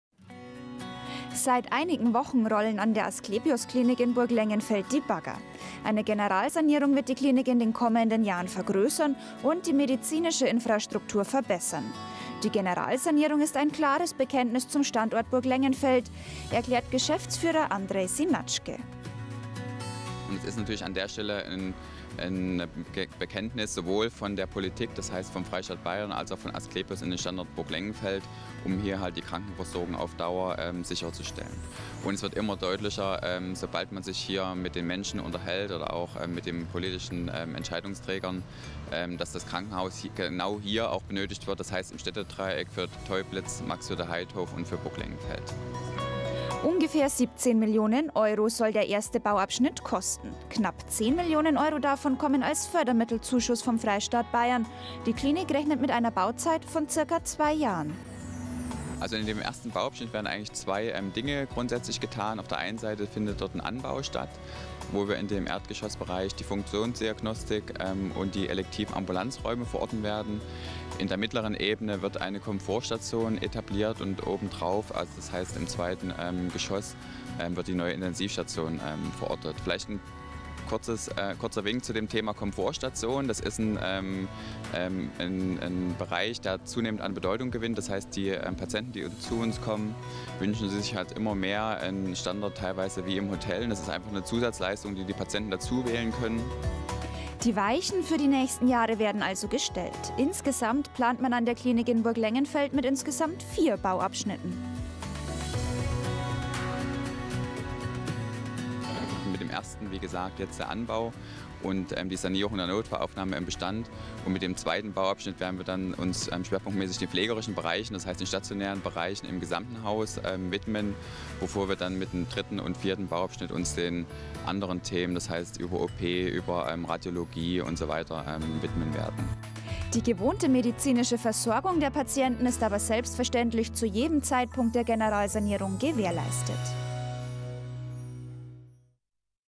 Klinikentwicklung
An der Asklepios Klinik Burglengenfeld geht viel voran. Unter anderem steht in den nächsten Jahren eine Generalsanierung an. Geschäftsführer Andrej Synnatzschke informiert über die Planungen.